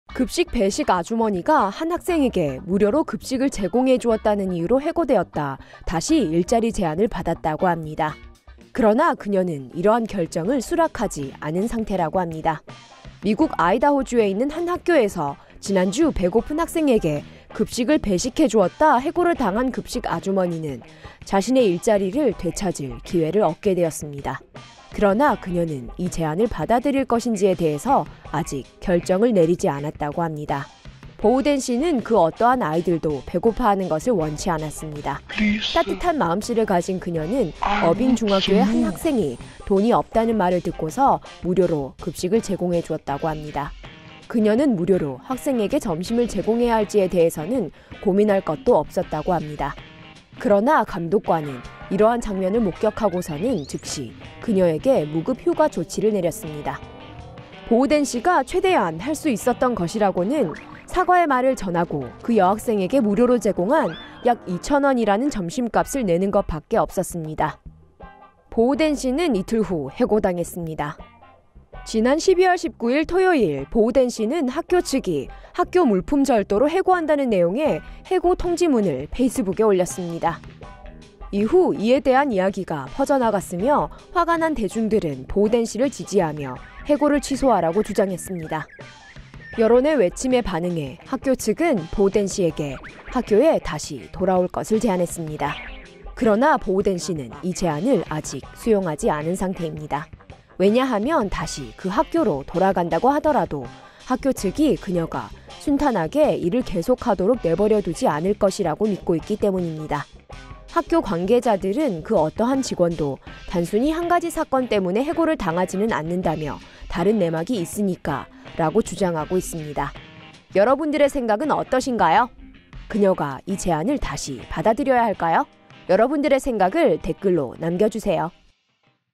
무료급식을 제공했다 쫓겨난 급식 아주머니, 해고 철회 제안받았으나, 거절
미국, 아이다호, 포카텔로 — 급식 배식 아주머니가 한 학생에게 무료로 급식을 제공해주었다는 이유로 해고되었다, 다시 일자리 제안을 받았다고 합니다. 그러나 그녀는 이러한 결정을 수락하지 않은 상태라고 합니다.

미국, 아이다호 주에 있는 한 학교에서 지난 주 배고픈 학생에게 급식을 배식해주었다 해고를 당한 급식 아주머니는 자신의 일자리를 되찾을 기회를 얻게 되었습니다. 그러나 그녀는 이 제안을 받아들일 것인지에 대해서 아직 결정을 내리지 않았다고 합니다.

보우덴 씨는 그 어떠한 아이들도 배고파하는 것을 원치 않았습니다. 따뜻한 마음씨를 가진 그녀는 어빙 중학교의 한 학생이 돈이 없다는 말을 듣고서 무료로 급식을 제공해주었다고 합니다.

그녀는 무료로 학생에게 점심을 제공해야할지에 대해서는 고민할 것도 없었다고 합니다.

그러나 감독관은 이러한 장면을 목격하고서는, 즉시 그녀에게 무급 휴가 조치를 내렸습니다.

보우덴 씨가 최대한 할 수 있었던 거라고는 사과의 말을 전하고, 그 여학생에게 무료로 제공한 약 2천원이라는 점심값을 내는 것 밖에 없었습니다. 그러나 이러한 그녀의 제한은 무시당했다고 합니다.

그리고 그녀는 이틀 후, 해고당했습니다. 보우덴 씨가 지난 12월 19일 토요일, 학교 측이 보우덴 씨가 '학교 물품 절도'로 해고한다는 내용의  해고 통지문을 페이스북에 올렸습니다.

이후 이에 대한 이야기가 퍼져나갔고, 화가 난 대중들은 보우덴 씨를 지지하며, 해고를 취소하라고 주장했습니다.

여론의 외침에 반응해, 학교 측은 보우덴 씨에게 학교에 다시 돌아올 것을 제안했습니다.

그러나 보우덴 씨는 이 제안을 아직 수용하지 않은 상태입니다. 왜냐하면, 다시 그 학교로 돌아간다고 하더라도, 학교측이 그녀가 순탄하게 일을 계속하도록 내버려두지 않을 것이라고 믿고 있기 때문입니다.

학교 관계자들은 그 어떠한 직원도 단순히 한가지 사건때문에 해고를 당하지는 않는다며, 다른 내막이 있으니까라고 주장하고 있습니다.

여러분들의 생각은 어떠신까요? 그녀가 이 제안을 다시 받아들여야 할까요? 여러분들의 생각을 댓글로 남겨주세요.